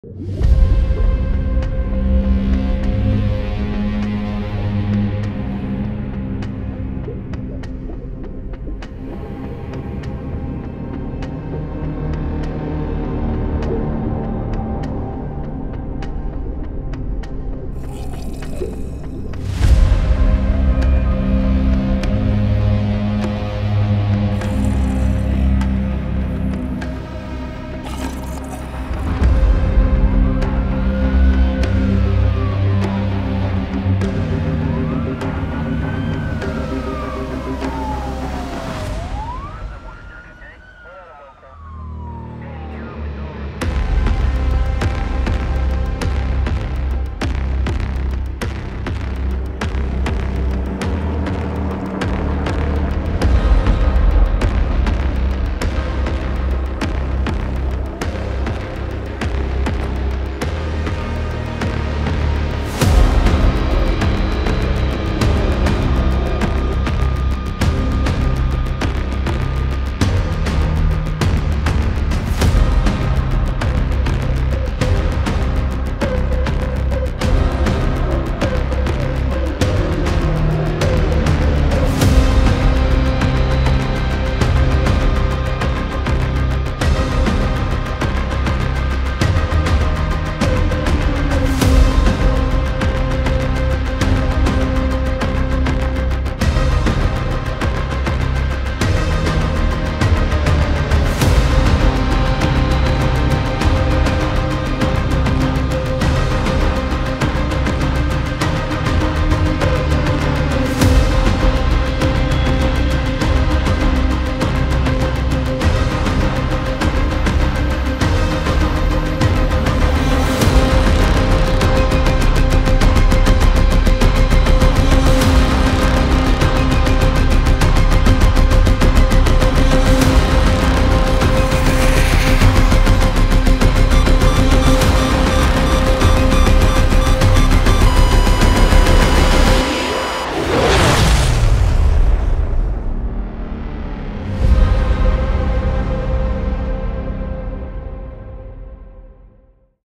Infected Earth | Zombie Apocalypse Trailer
"Infected Earth," a spine-chilling zombie apocalypse trailer crafted with cutting-edge generative AI tools. Discover how AI is pushing the boundaries of creativity and storytelling like never before.

Licensed soundtrack- produced by Kevin Maison

@RunwayML @HaiperAI @lumalabsai & Midjourney